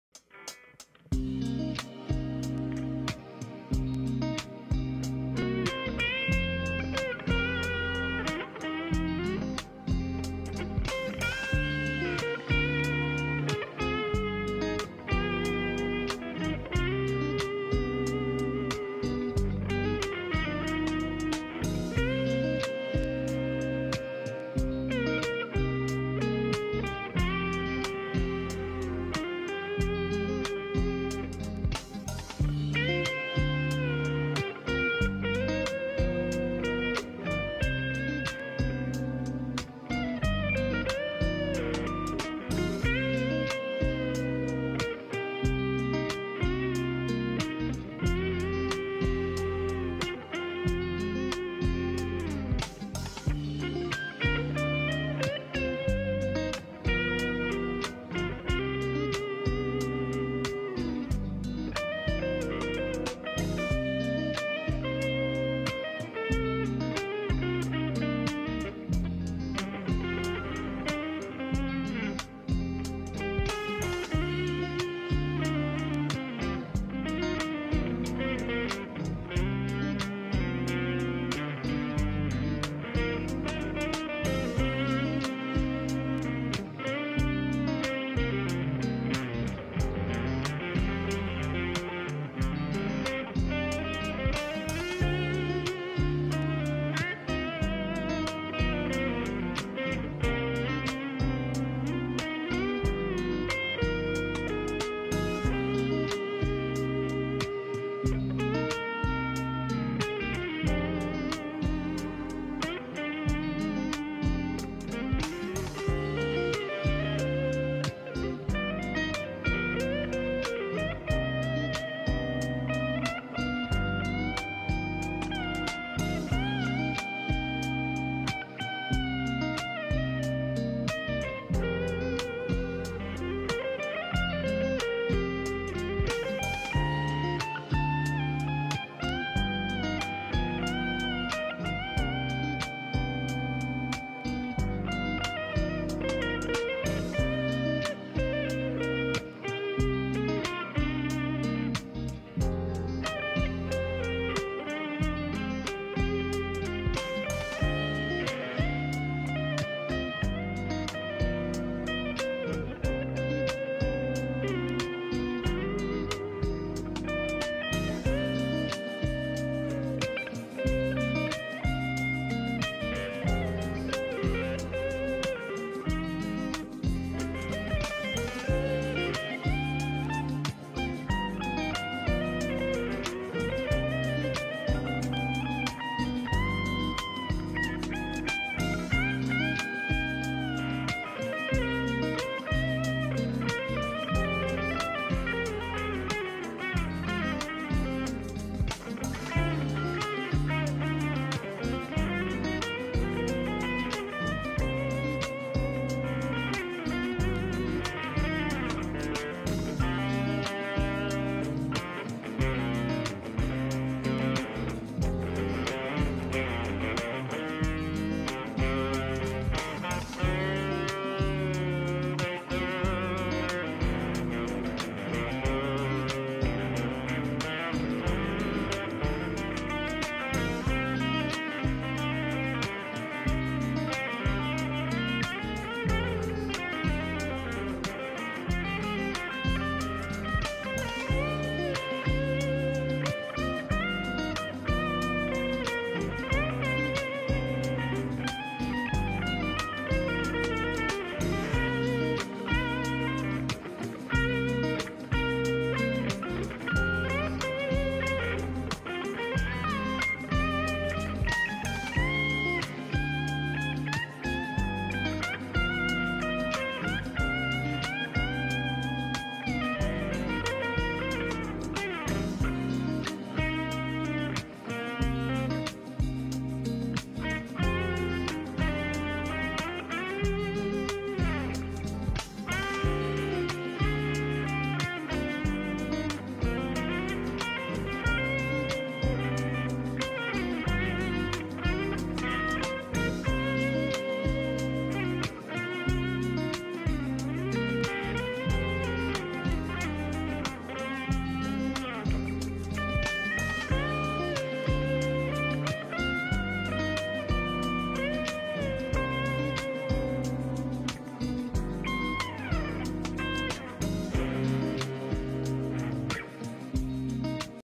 Telecaster with Zendrive by Aion
Noodlin with my Fender American Telecaster
loaded with Bareknuckle Pile Drivers

Amp: Marshall DSL1H
Efx: Azimuth Zendrive by Aion Electronics
Recorded through Roland GoMixer and Iphone6